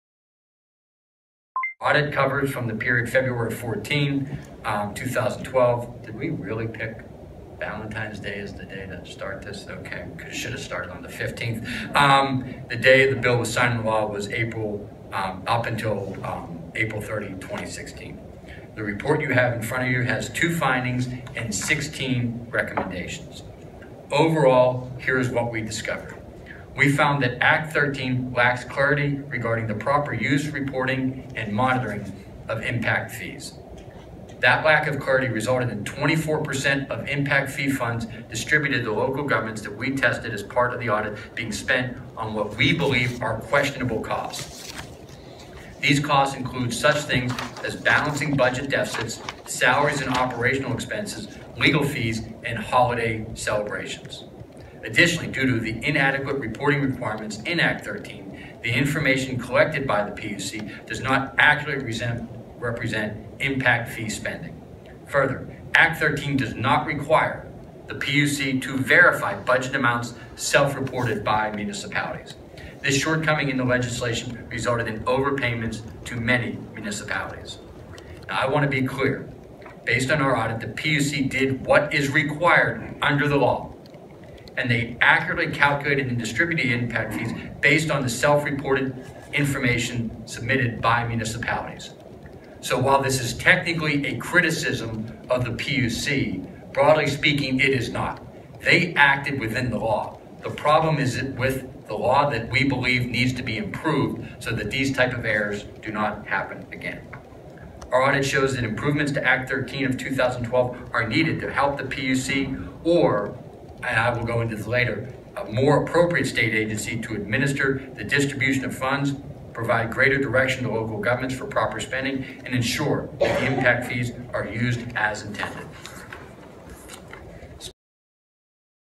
Auditor General Eugene DePasquale Discusses Deficiencies In Act 13
Auditor General Eugene DePasquale discusses audit that found lack of oversight of Act 13 impact fees.